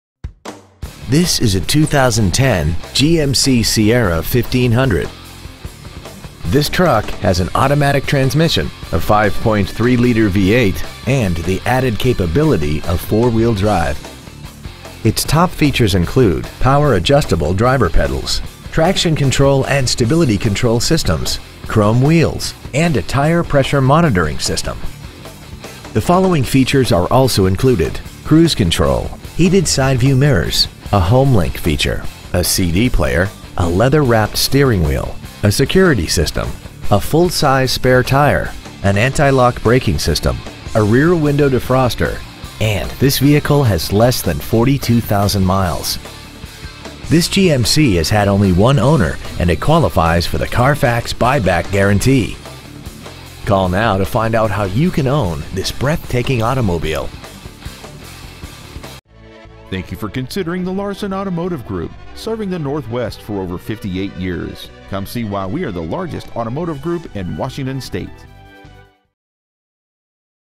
2010 GMC Sierra 1500 Puyallup WA
Year : 2010
 Make : GMC
 Model : Sierra 1500
 Engine : 5.3L V8 OHV 16V FFV
 Trans . : Automatic
 Exterior : Black
 Miles : 41,347
 Interior : Black
 Stock : A4286A

 Preowned 2010 GMC Sierra 1500 Puyallup WA